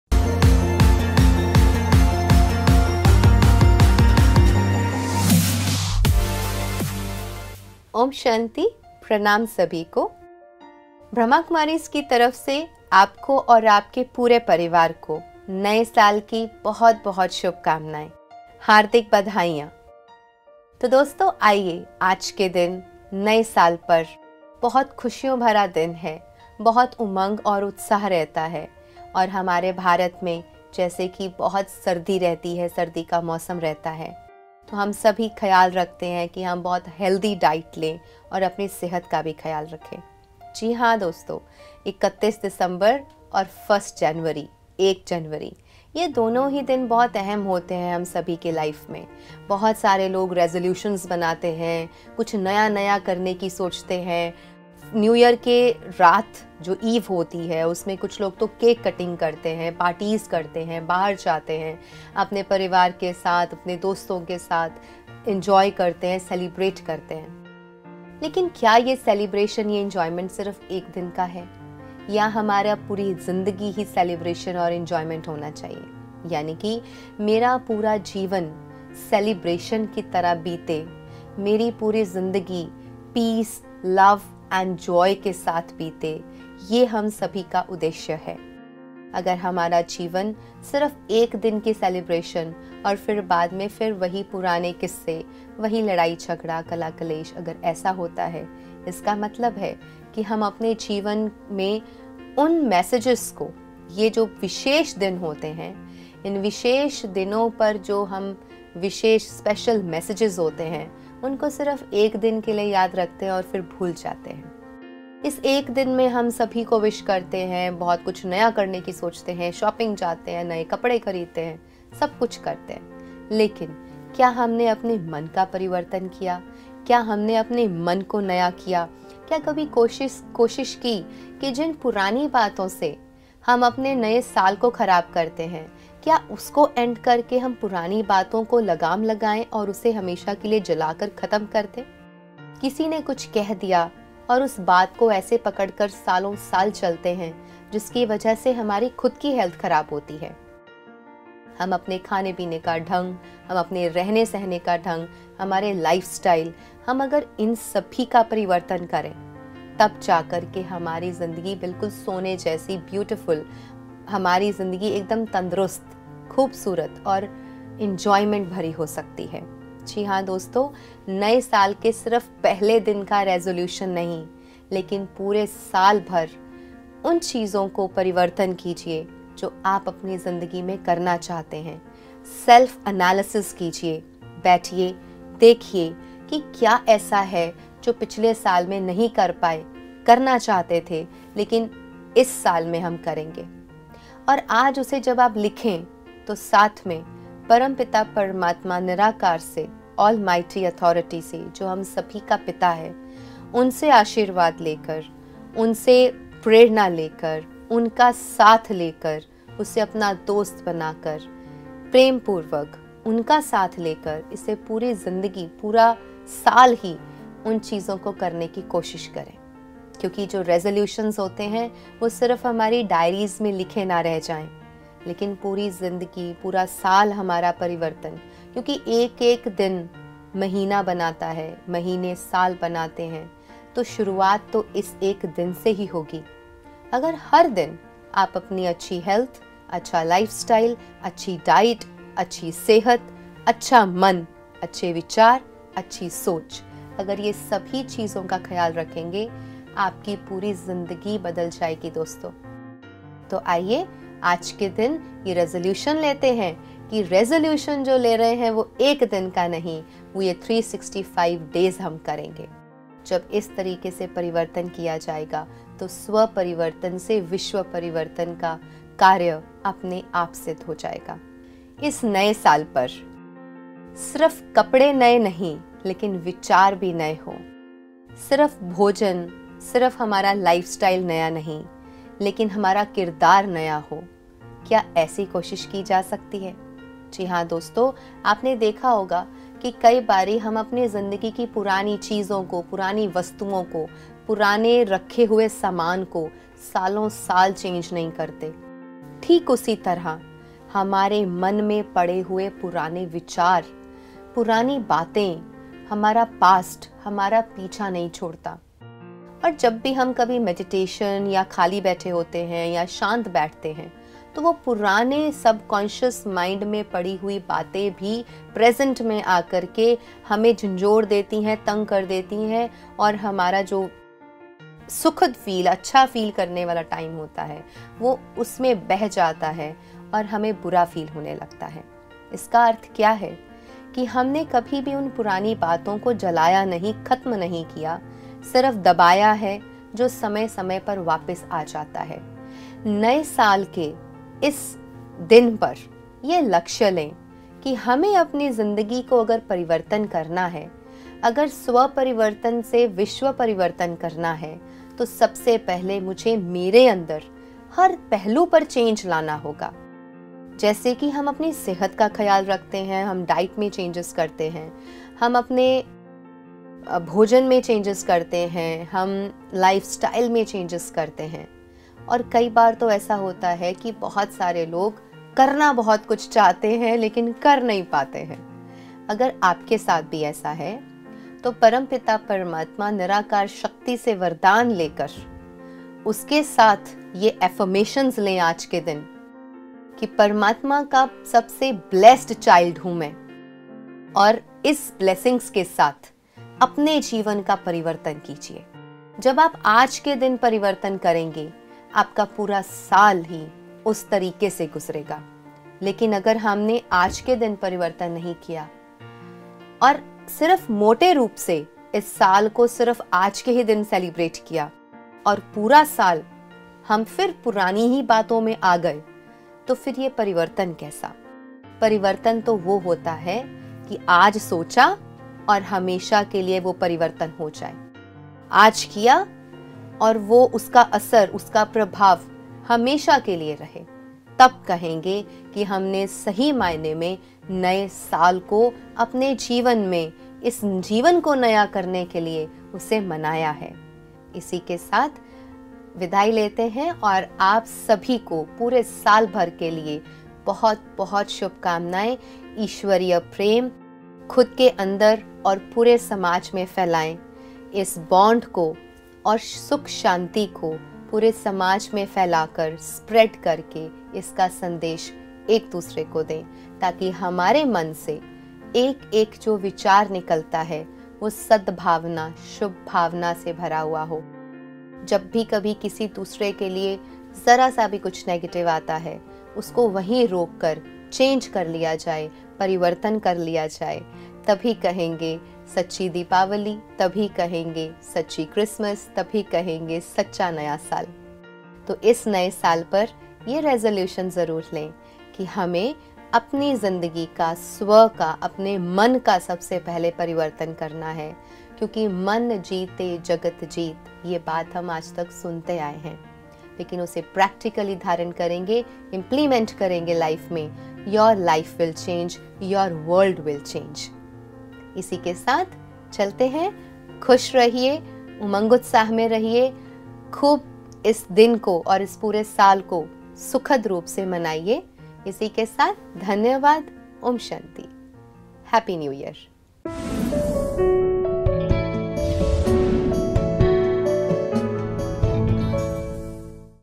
क्या नए साल में 1 दिन का CELEBRATION ही पर्याप्त है? New Year 2024 |BK Karamjeet Sister
Organizer: Brahmakumaris